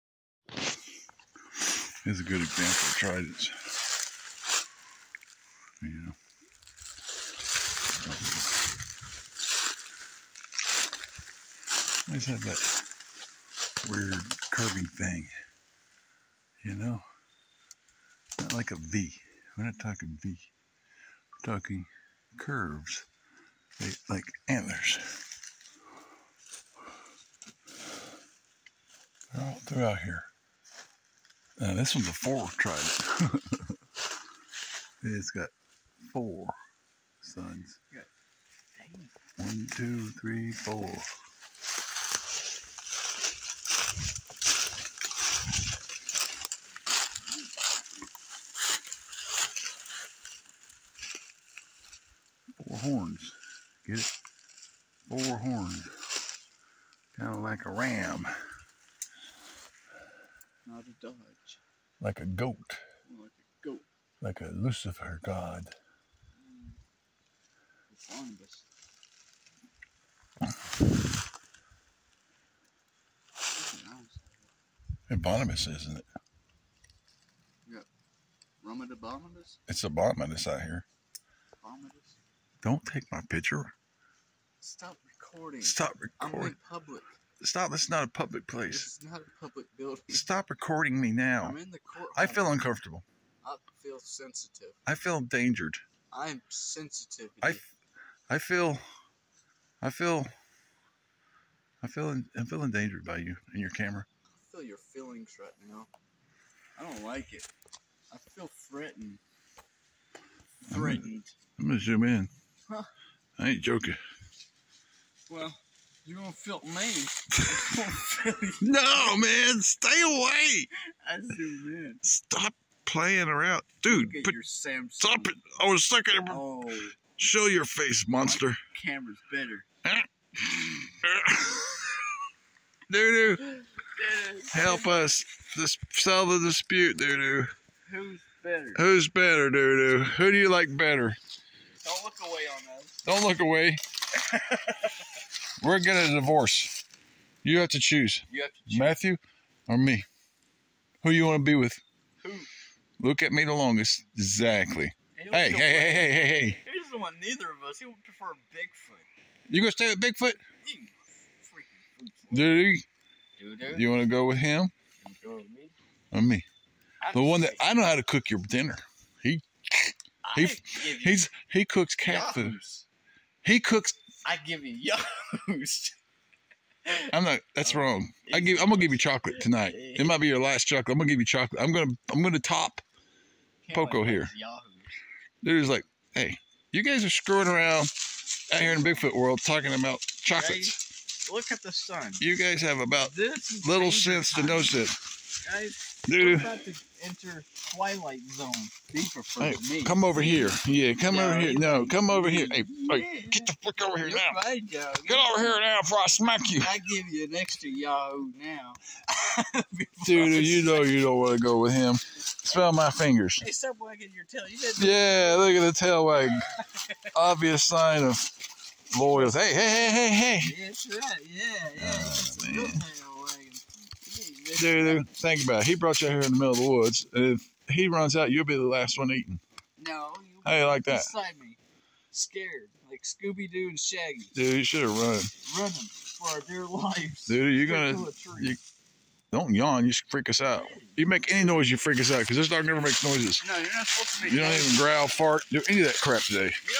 Trident and Matt and I playing around.
Lots of hunters and not much activity this weekend